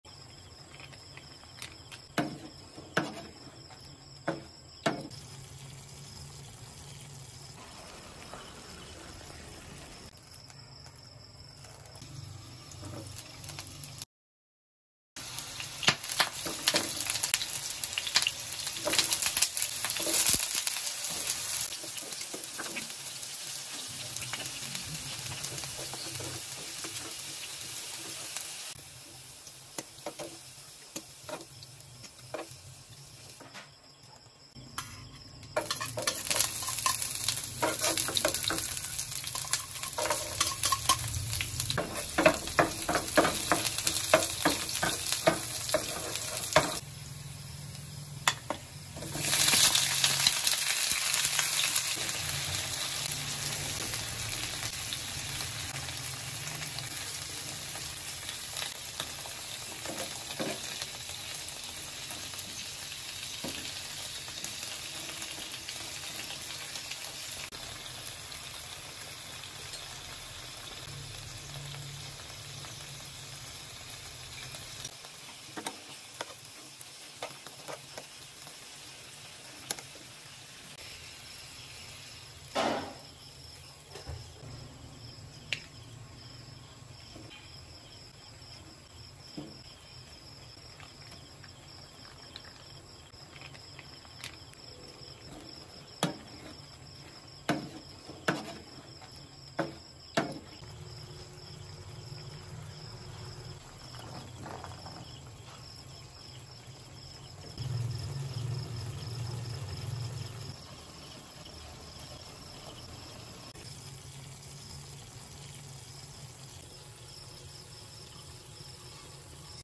KALDERETANG HIPON
Ibahin mo naman ang pagluto sa hipon. Gawin mo itong kaldereta para maiba naman ang lasa. Tiyak mapapa-unli rice ka sa pagkain nito!